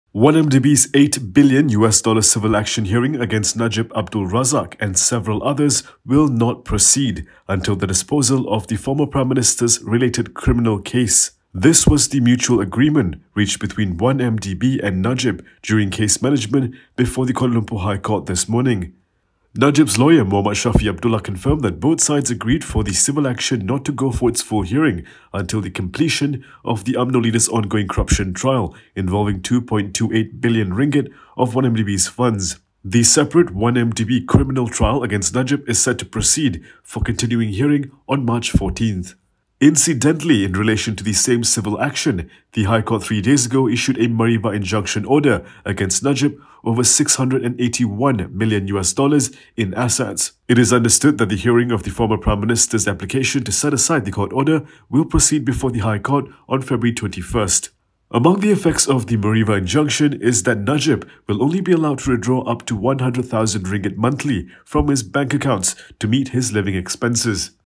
1MDB, Najib agree to temporary hold on US$8b lawsuit
Sovereign wealth fund 1MDB's US$8 billion (RM33.5 billion) civil action hearing against Najib Abdul Razak and several others will not proceed until the disposal of the former prime minister's related criminal case.

This was the mutual agreement reached between 1MDB and Najib, who was the former chairperson of the fund's board of advisors, during case management before the Kuala Lumpur High Court this morning.